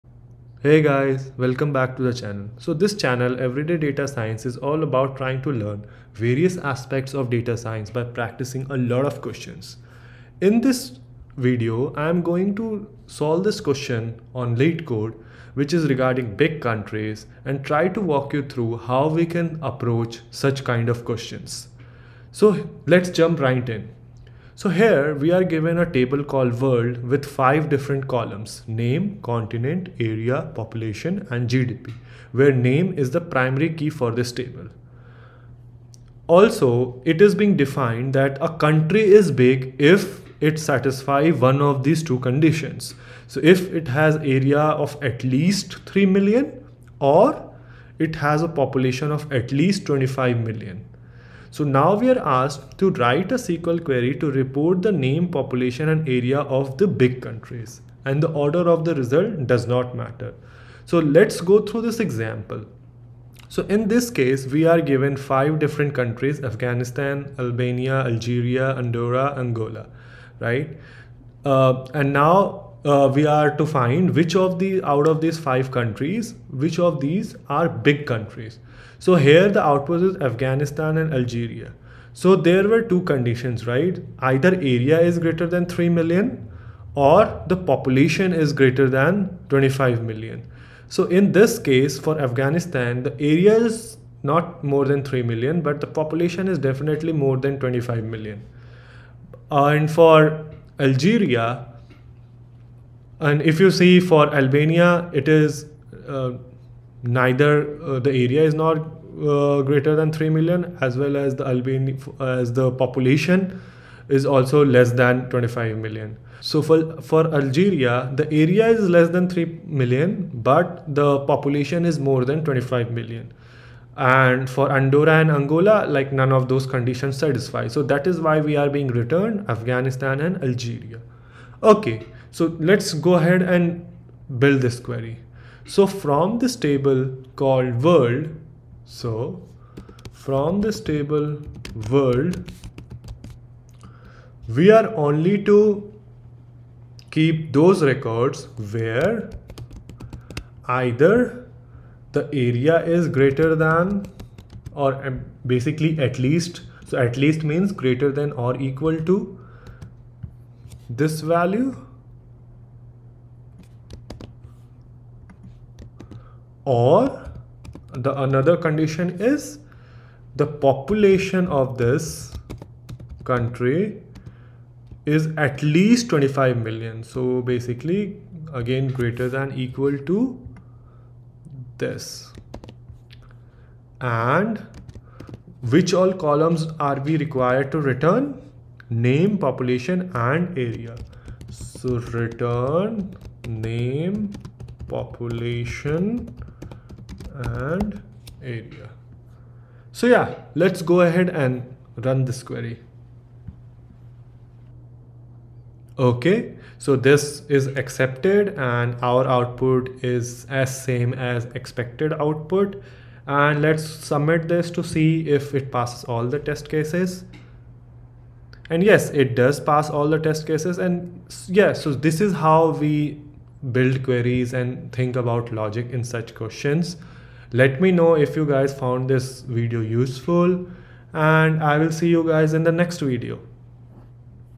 LeetCode Interview SQL Question with Detailed Explanation | Practice SQL | LeetCode 595
Previous Video:  LeetCode 586 Customers With Maximum Orders

In this video I solve and explain a leetcode SQL question using MySQL query. This question has been asked in Apple, Facebook, Amazon, Google, Adobe, Microsoft, Adobe interviews or what we popularly call FAANG interviews.
I explain the related concept as well. This question is about finding the big countries and also includes points to keep in mind to develop SQL queries. You will also learn about WHERE clause.